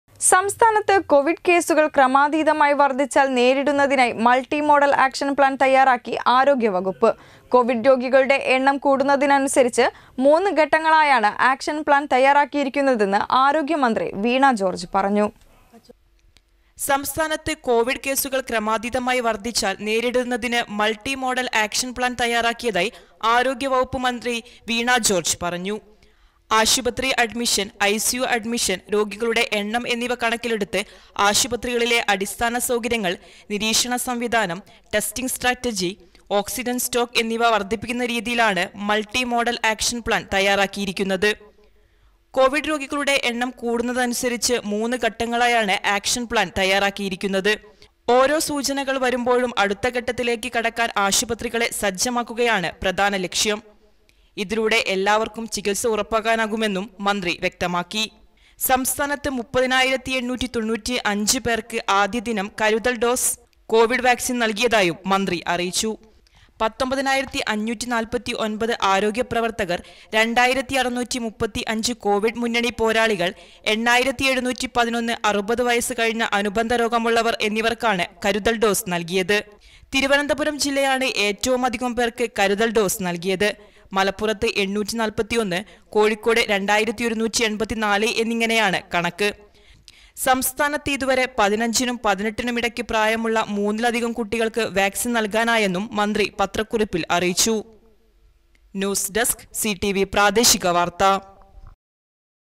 കോവിഡ് കേസുകൾ ക്രമാതീതമായി വർധിച്ചാൽ നേരിടുന്നതിനായി മൾട്ടി മോഡൽ ആക്ഷൻപ്ലാൻ തയ്യാറാക്കിആരോഗ്യവകുപ്പ്
സംസ്ഥാനത്ത് കോവിഡ് കേസുകൾ ക്രമാതീതമായി വർധിച്ചാൽ നേരിടുന്നതിനായി മൾട്ടി മോഡൽ ആക്ഷൻ പ്ലാൻ തയ്യാറാക്കി ആരോഗ്യവകുപ്പ്.കോവിഡ് രോഗികളുടെ എണ്ണം കൂടുന്നതനുസരിച്ച് മൂന്ന് ഘട്ടങ്ങളായാണ് ആക്ഷൻ പ്ലാൻ തയ്യാറാക്കിയിരിക്കുന്നതെന്ന് ആരോഗ്യ മന്ത്രി വീണ ജോർജ് പറഞ്ഞു